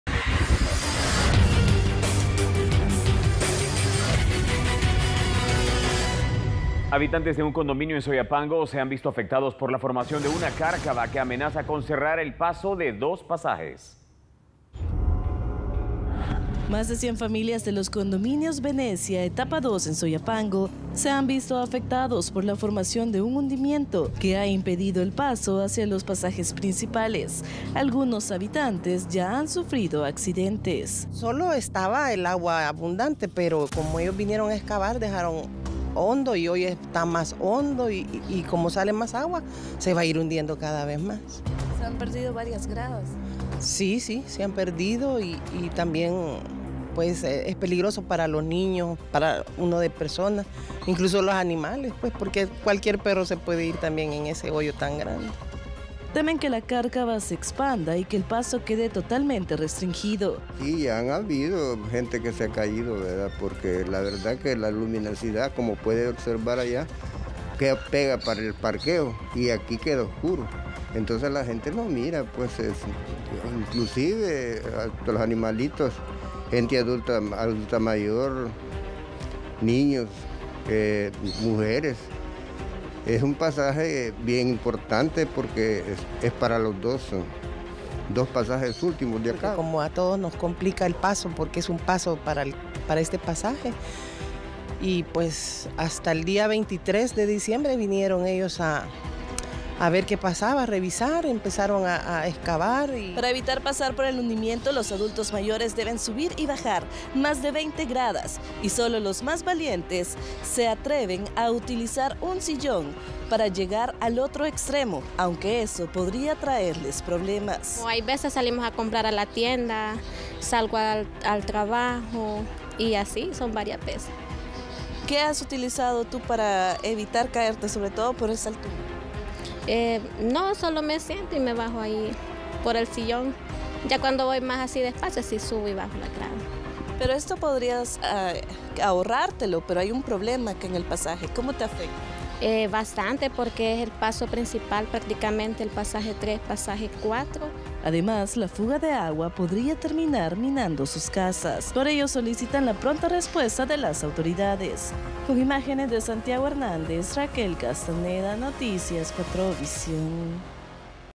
HUNDIMIENTO CONDOMINIO VENECIA
Habitantes de un condominio en Soyapango se han visto afectados por la formación de una cárcava que amenaza con cerrar el paso de 2 pasajes.